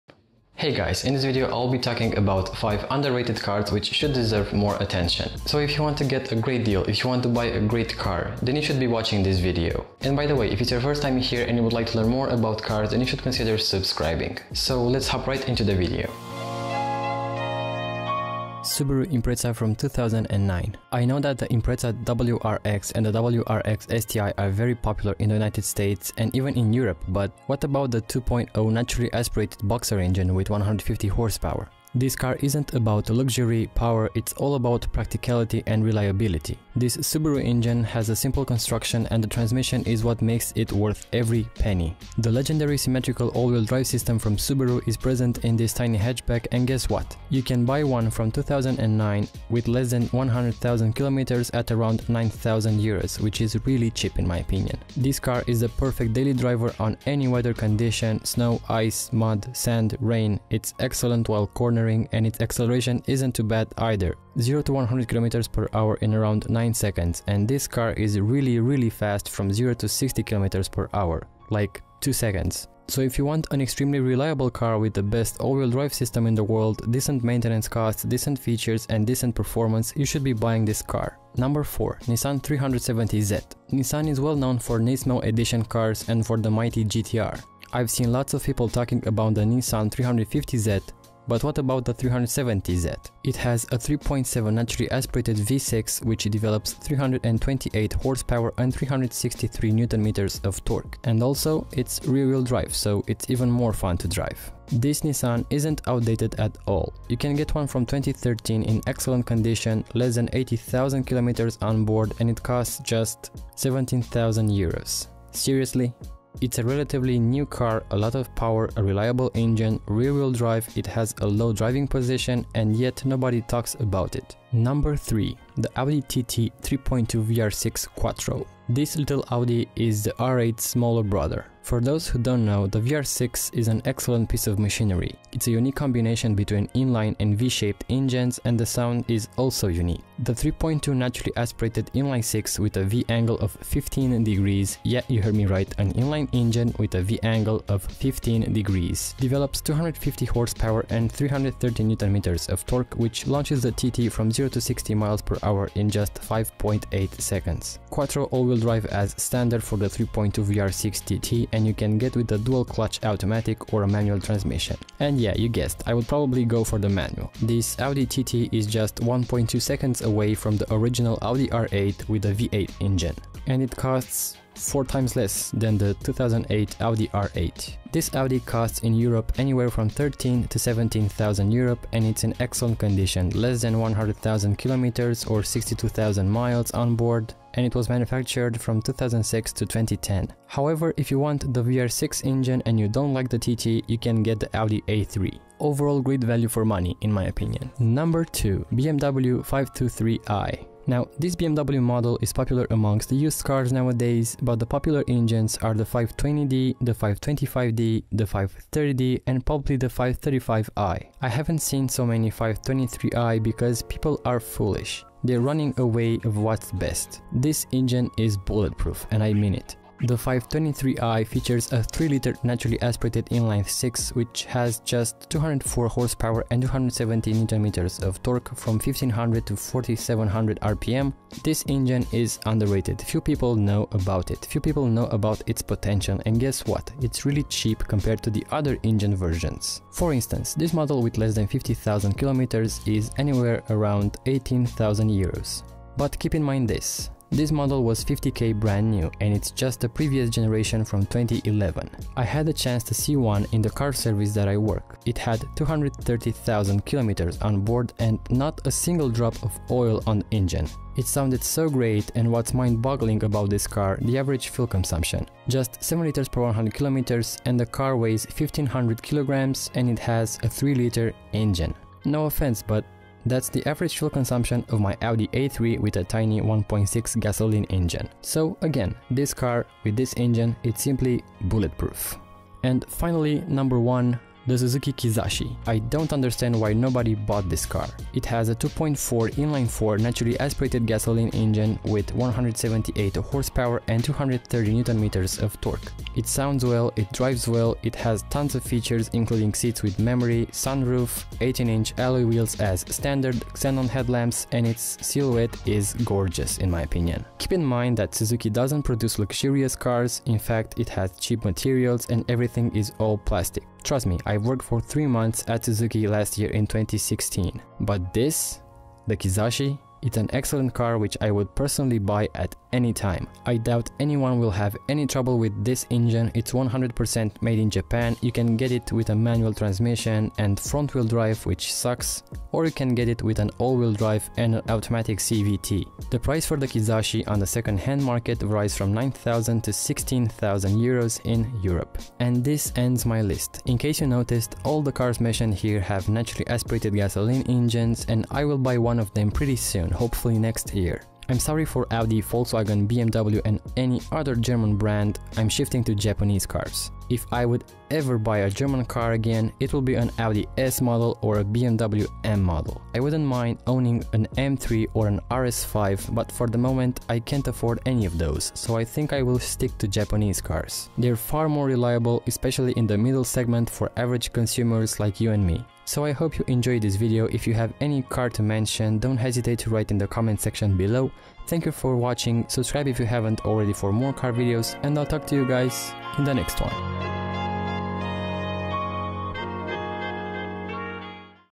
Why Are These Cars So Underrated???
If you're planning to get a great deal, then watch this video. If you're new to this channel, then welcome!

In this video I am talking about 5 cars which I believe are super underrated.

Find out how much they cost and which are those cars.